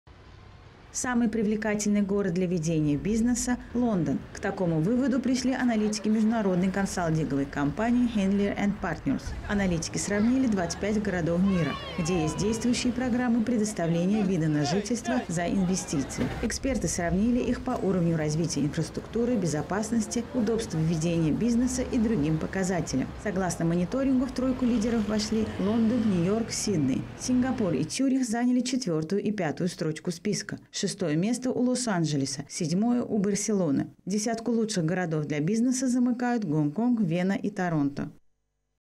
Лондон признали самым привлекательным для бизнеса
Самый привлекательный город для ведения бизнеса - Лондон. К такому выводу пришли аналитики международной консалтинговой компании Henley & Partners.  Аналитики сравнили 25 городов мира, где есть действующие программы предоставления вида на жительство за инвестиции. Эксперты сравнили их по уровню развития инфраструктуры, безопасности, удобству ведения бизнеса и другим показателям. Согласно мониторингу, в лидеры вошли Лондон, Нью-Йорк. Сидней и Цюрих заняли четвертую и пятую строчку списка. Шестое место у Лос-Анджелеса, седьмое — у Барселоны. Десятку лучших городов для бизнеса замыкают Гонконг, Вена и Торонто.